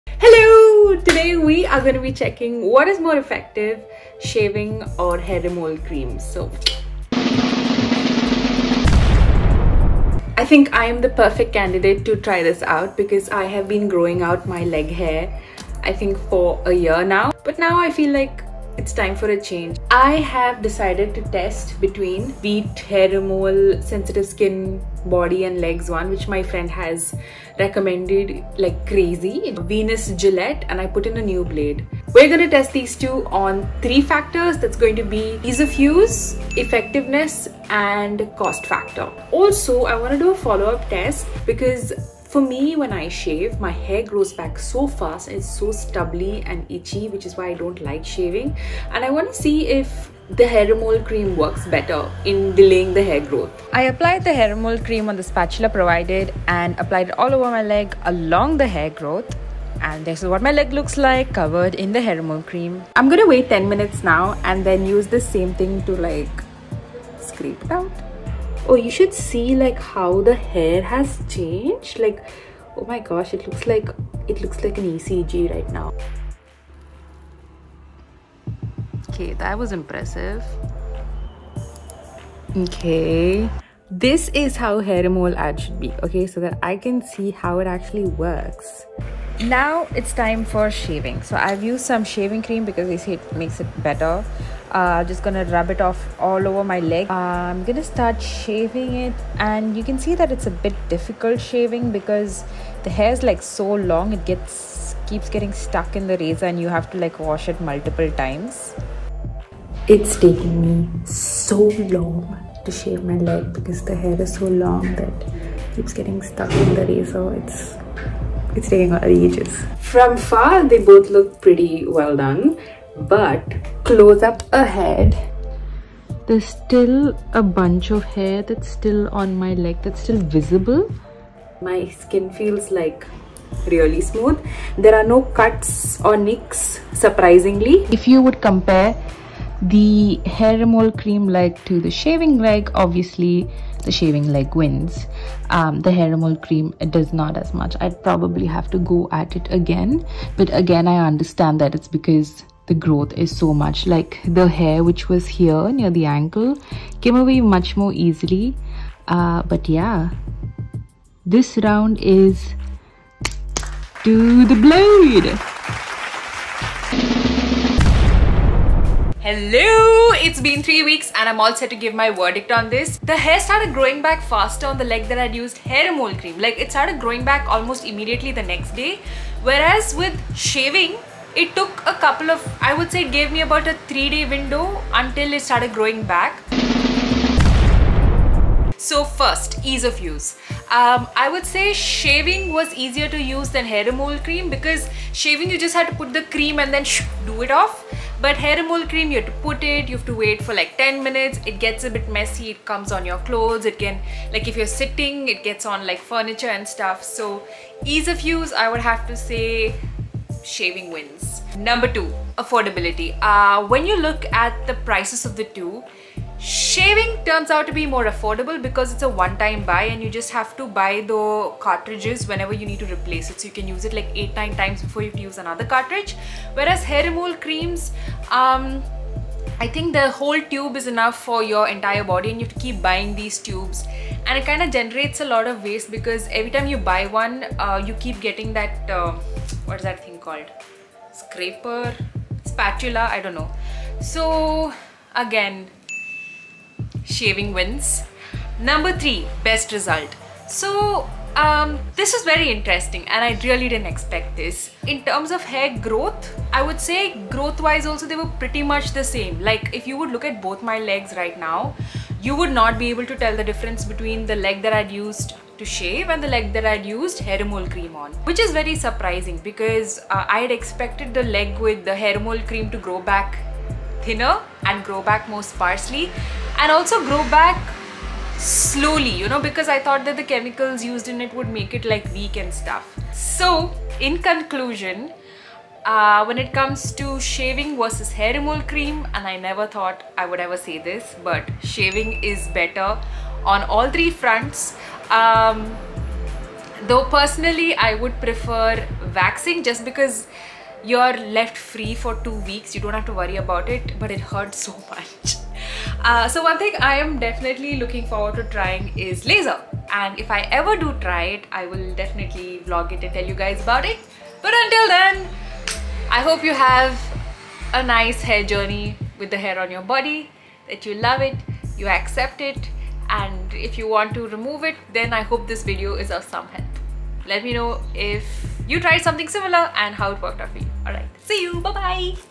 testing whether shaving or hair removal cream is better for women | + 3 week review | satshyaa
i always wondered about this so decided to do a proper test. i've used the veet hair removal cream for sensitive skin and the gillette venus razor and then did a review after 3 weeks. i'm sure everyone's hair is different so this wouldn't be applicable to all women in india but this is my personal observation over a period of 3 weeks. hope it helps!